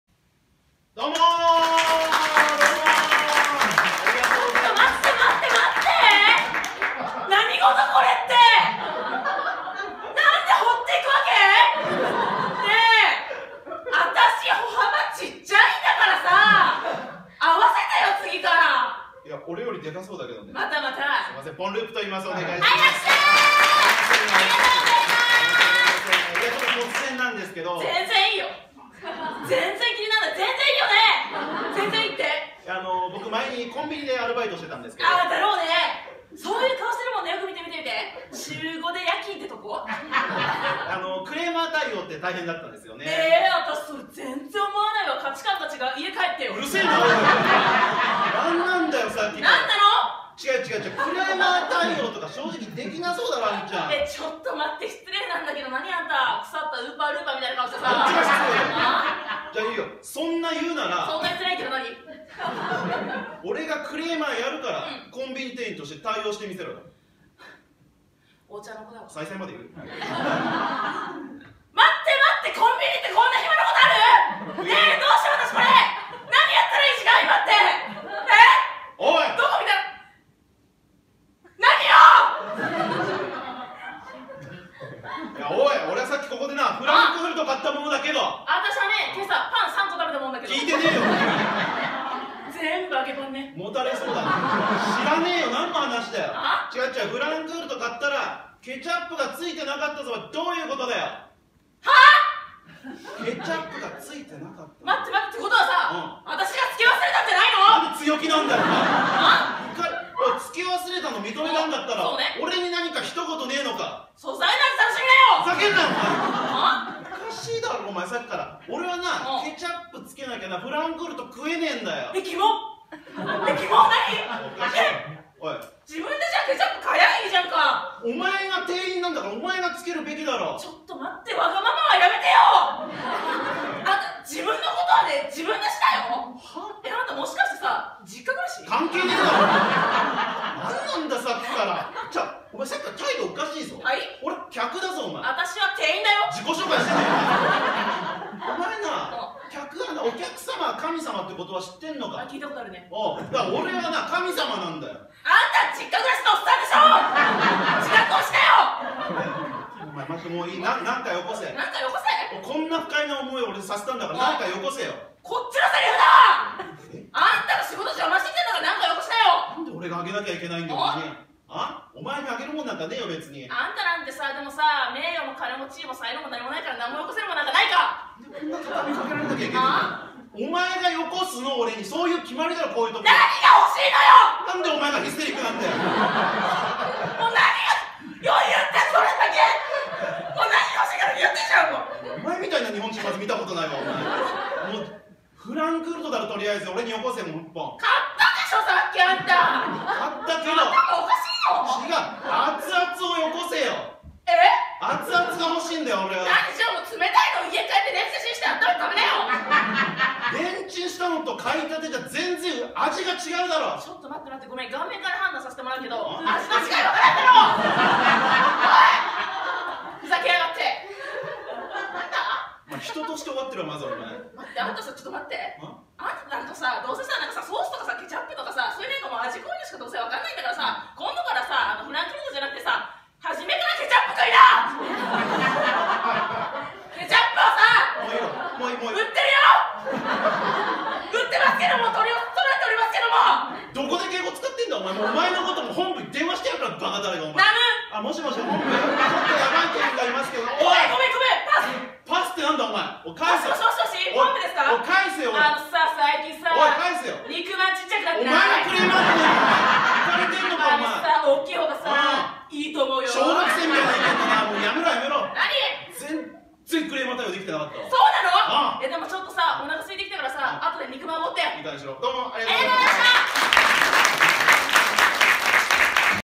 No.25：ポンループ アミ【やついフェスキャンペーンガール2021応募者】
身長173センチ、体重73キロという元中日の井端選手と全く同じ体型を活かして、がんばります！！